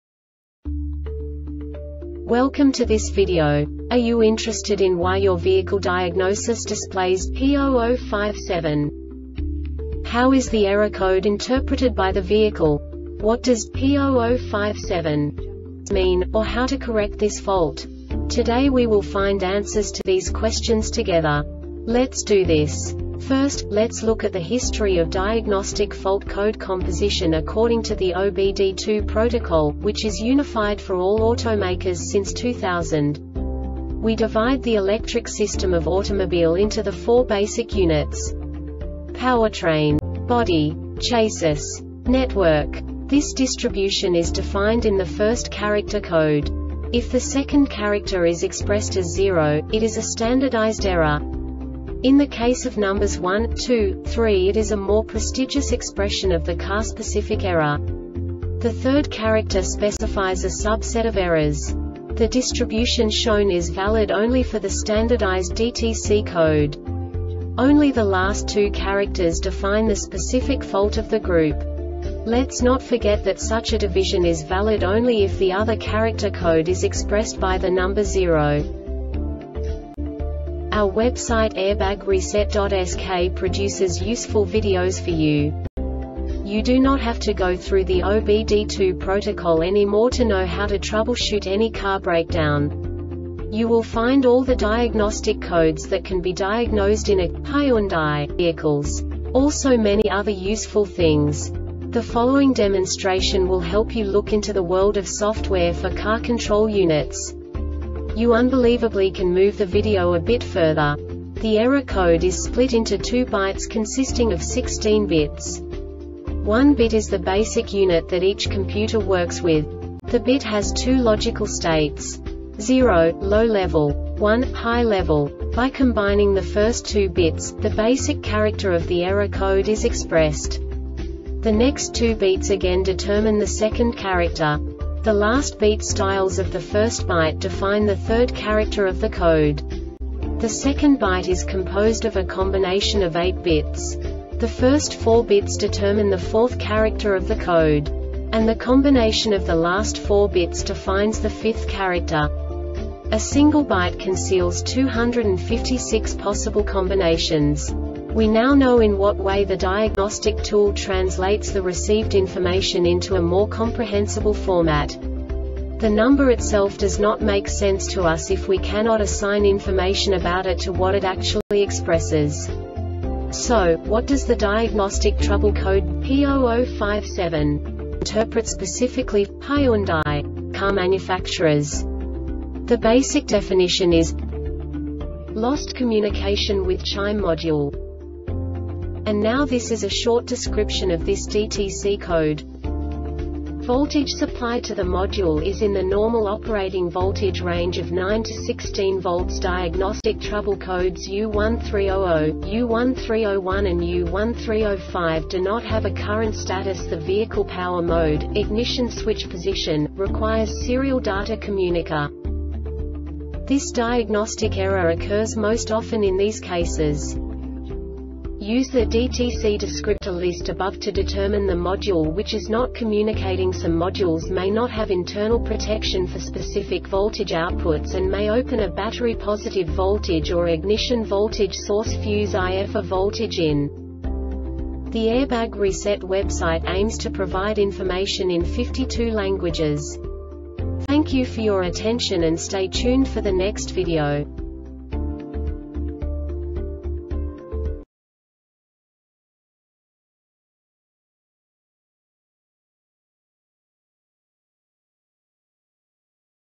DTC Hyundai P0057 Short Explanation
Contents:
0:21 Basic DTC analysis according to OBD2 protocol standard.
1:48 Insight into programming
2:58 A diagnostically understandable description of the Diagnostic Trouble Code
3:05 Basic error definition

Make:
Hyundai
Code:
P0057
Definition:
Lost Communication With Chime Module
Description: